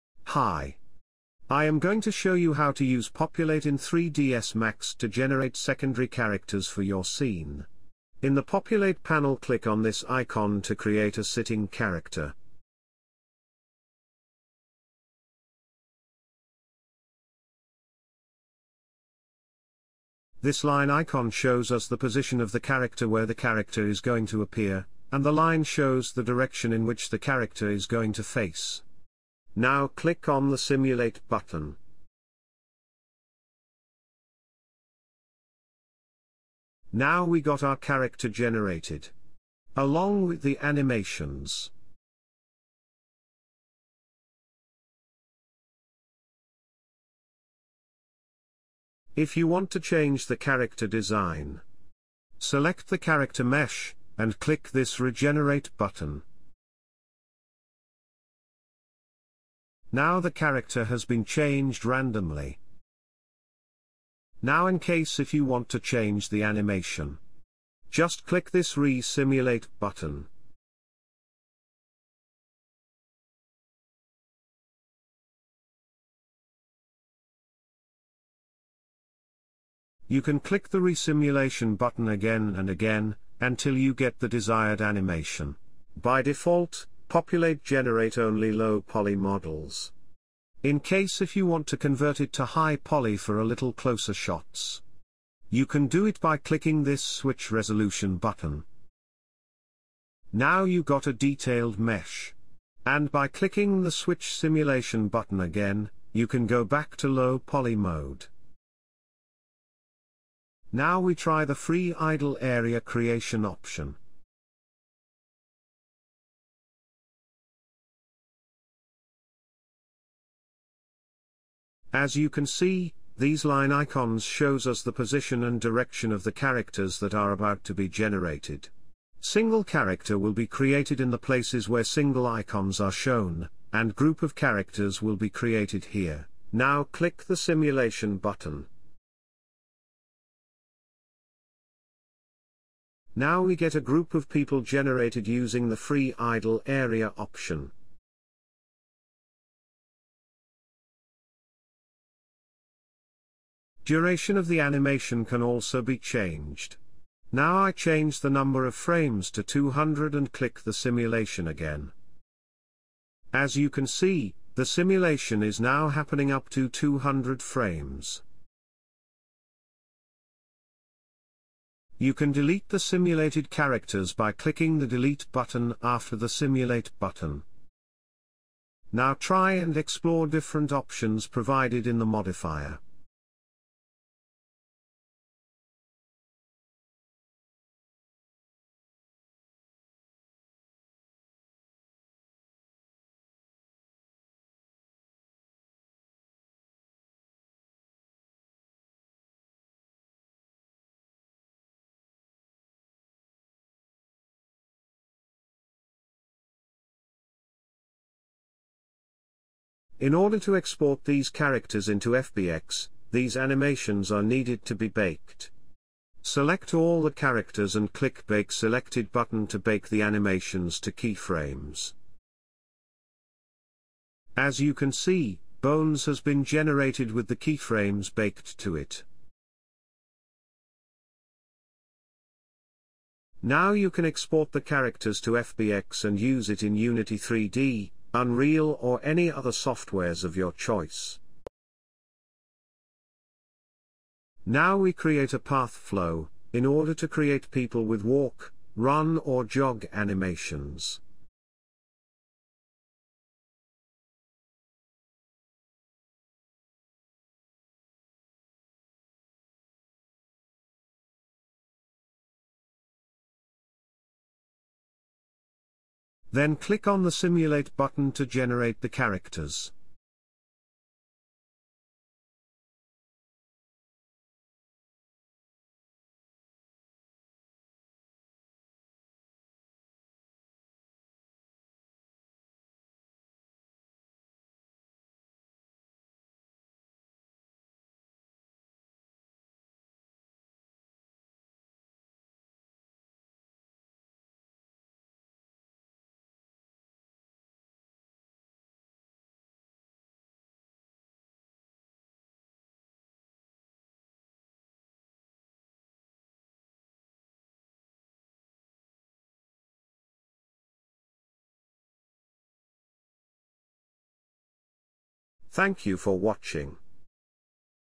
Generating multiple 3d characters in 3dsmax using Populate || Creating people crowd in 3dsmax
Populate is a plugin for 3dsmax available in Autodesk store.